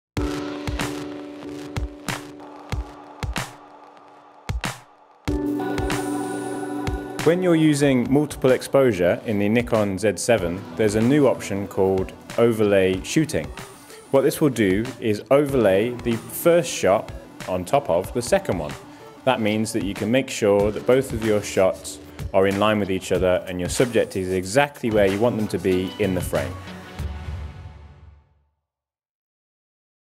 Nikon Z Tips: Overlay Shooting
We talk you through the new feature in the Z series called overlay shooting, allowing you to overlay two shots ensuring both images are perfectly in line. Find out more about the Nikon Z series